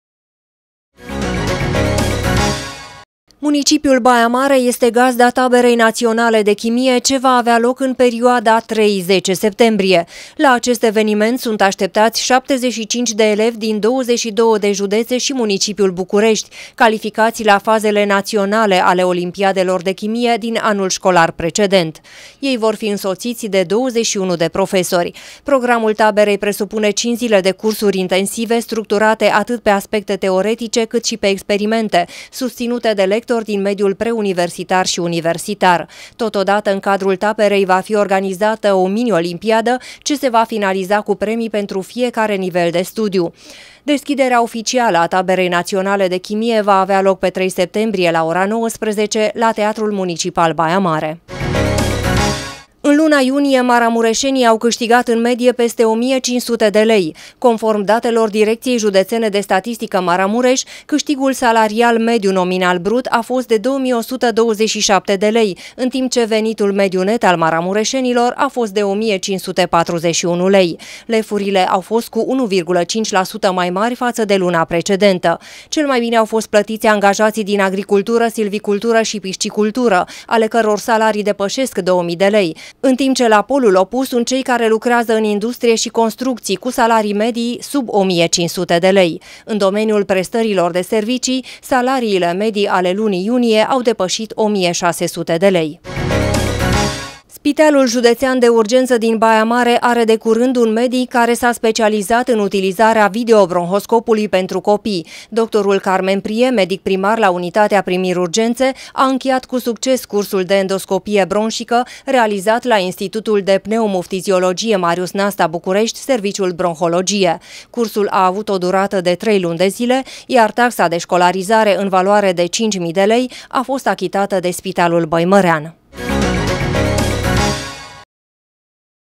ȘTIRI PE SCURT
Spitalul judetean are un SPECIALIST ÎN UTILIZAREA BRONHOSCOPULUI  Aflati mai multe detalii la stirile pe scurt.